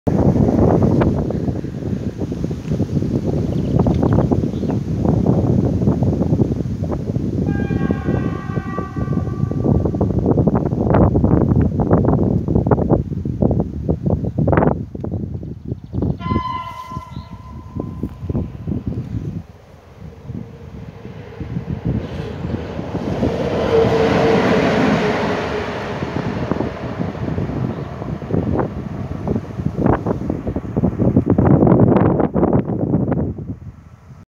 Lokomotywa elektryczna | ET22-977 PKP Cargo
Przejazd lokomotywy elektrycznej ET22, należącej do PKP Cargo, na przejeździe kolejowym w Rybniku-Gotartowicach.